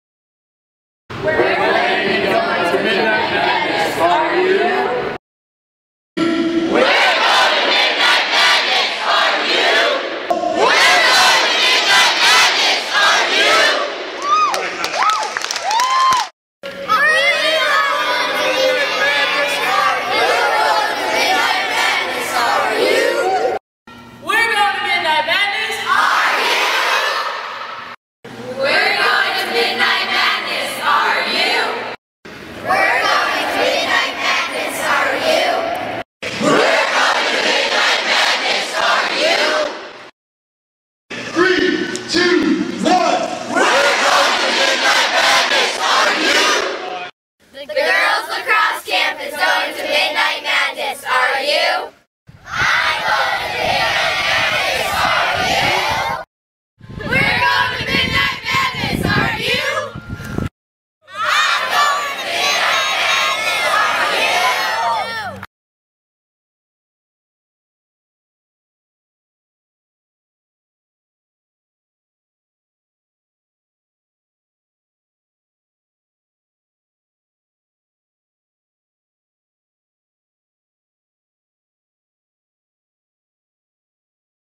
QU Midnight Madness Promos - Updated 9-8-11
Quinnipiac Students and Campers Promote Midnight Madness 2011.